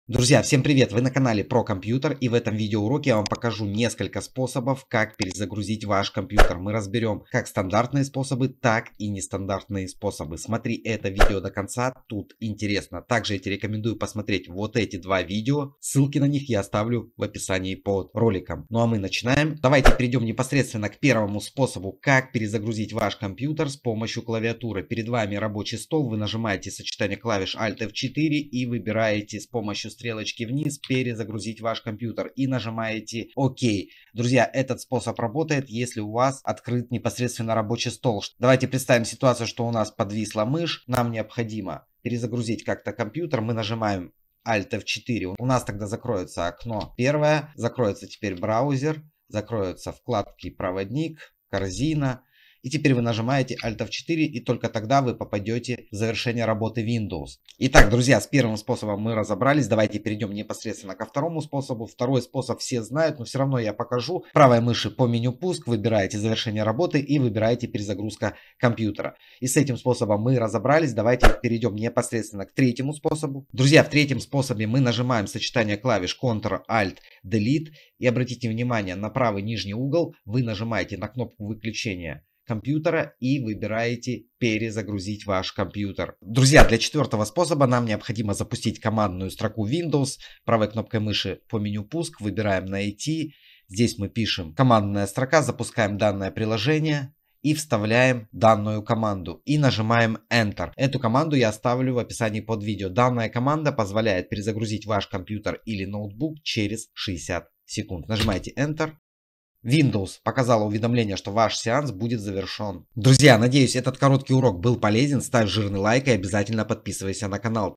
ТОП способы! Как перезагрузить компьютер стандартными и нестандартными способами!
«Как перезагрузить Компьютер» способами которые ты знаешь и которые ты будешь знать!  Если твой компьютер завис, или не работает мышка можно перезагрузить ваш компьютер с помощью клавиатуры. Я покажу 4 способа перезагрузки компьютера!
Перезагрузка Windows через командную строку через 60 сек
Откройте командную строку и введите следующую команду
shutdown /r
Главы:
00:00 - 4 Способа перезагрузки Windows
00:19 - 1 Способ
00:57 - 2 Стандартный Способ
01:11 - 3 Способ
01:26 - 4 Способ
✅ См. Также